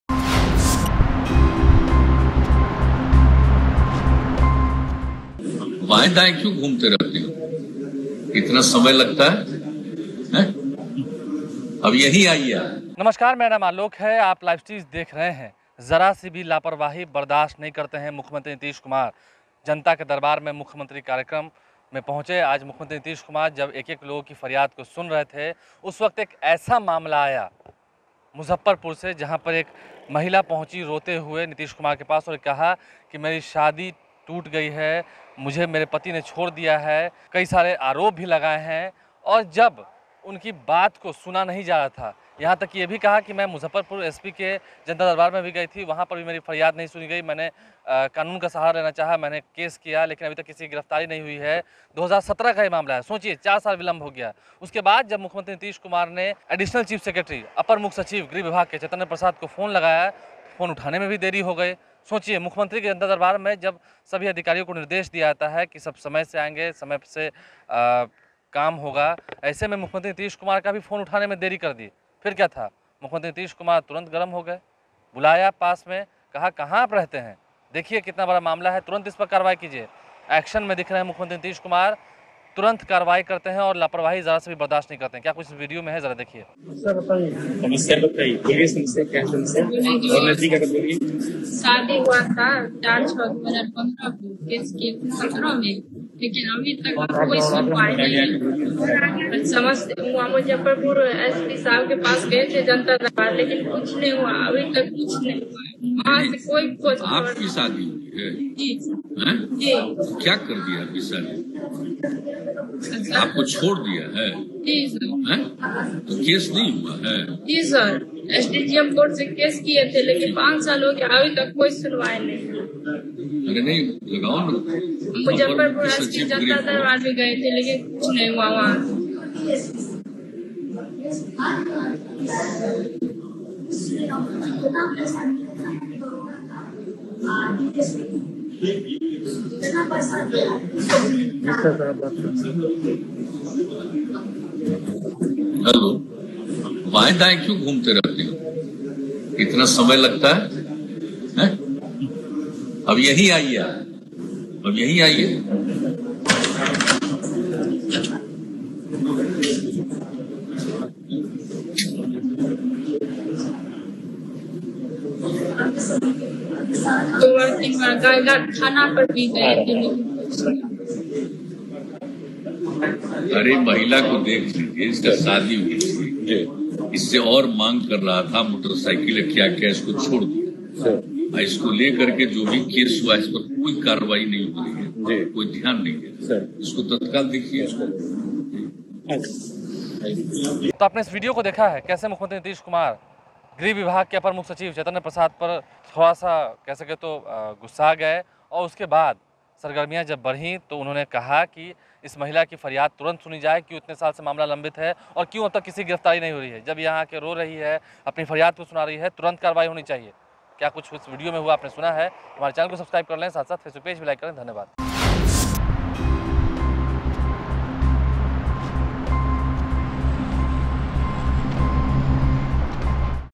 Janta Darbar में पहुंची एक महिला रोने लगी, CM Nitish अधिकारी पर भड़क गए, कहा- तुरंत कार्रवाई कीजिए
Janta Darbar में पहुंची एक महिला रोने लगी, CM Nitish अधिकारी पर भड़क गए, कहा- तुरंत कार्रवाई कीजिए | Bihar News | LiveCities